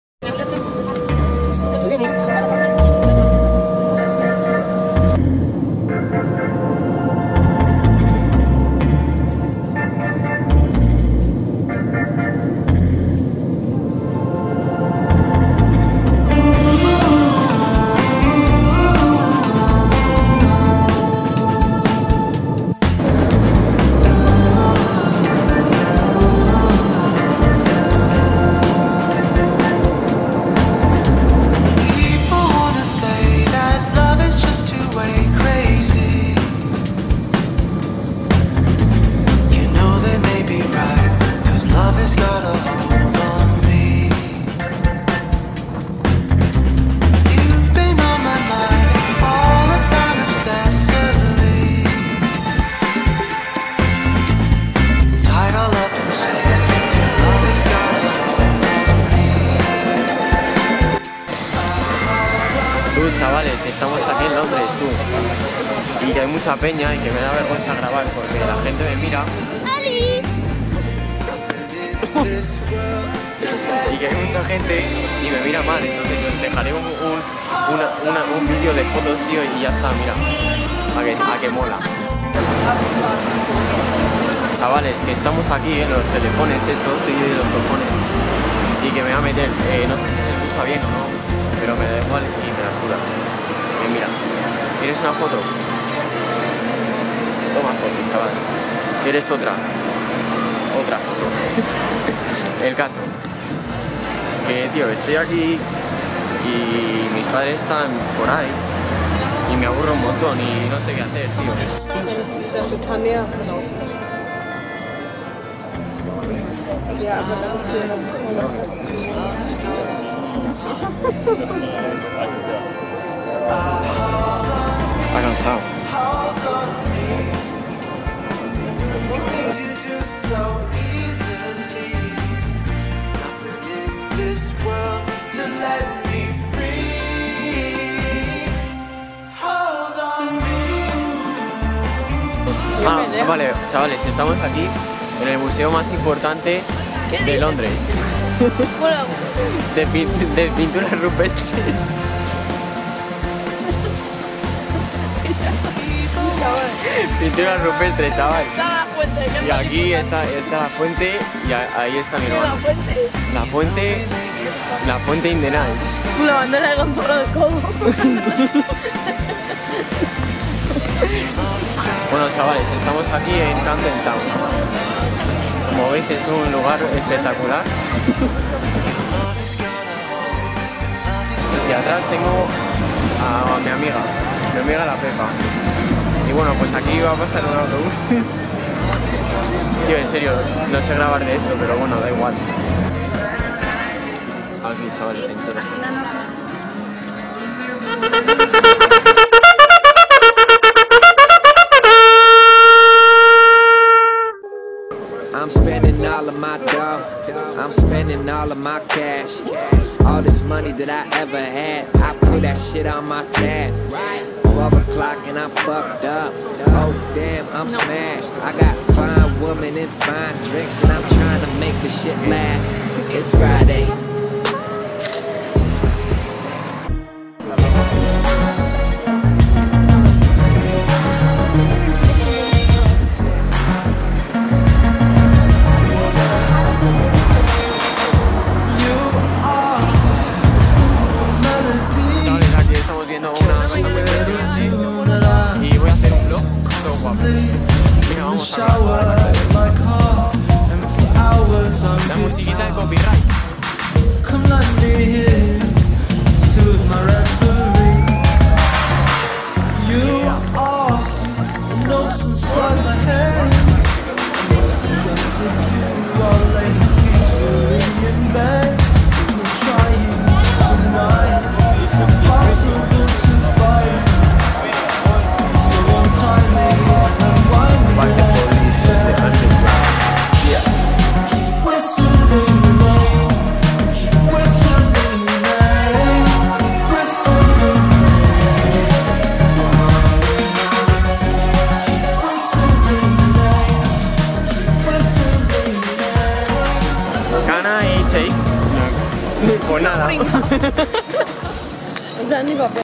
#1 Vlog en Londres !! Increiiiblee
Muy buenas chicos y chicas , bienvenidos un dia mas a mi canal hoy os dejo mi primer vlog en Londres y se que el vocabulario no es el que utlizo nunca es porque estaba un poco nervioso :)
Espero que os guste , darle a like y subscribiros
Este video esta grabado en :

▔▔▔▔▔▔╲▂▕▂▂▂I